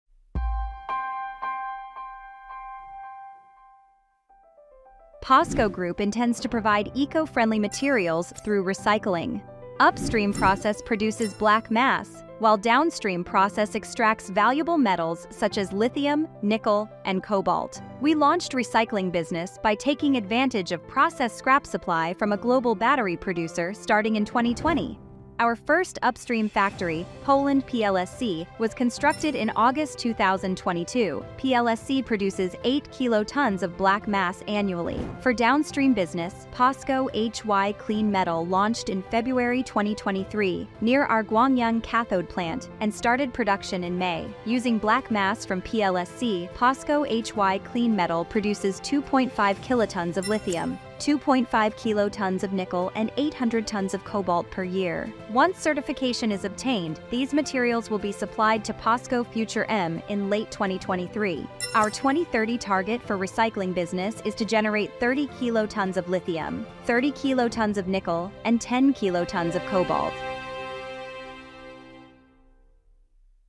How POSCO Holdings prepares for RECYCLING, the eco-friendly raw materials business | Value Day #03
Recycling business is an effort that companies can make for a sustainable world. POSCO Holdings has built both upstream and downstream processes for recycling, and POSCO HY Clean Metal is consistently producing valuable metals like lithium, nickel and cobalt. Watch the video to learn about POSCO Holdings' growth plans for the recycling business now!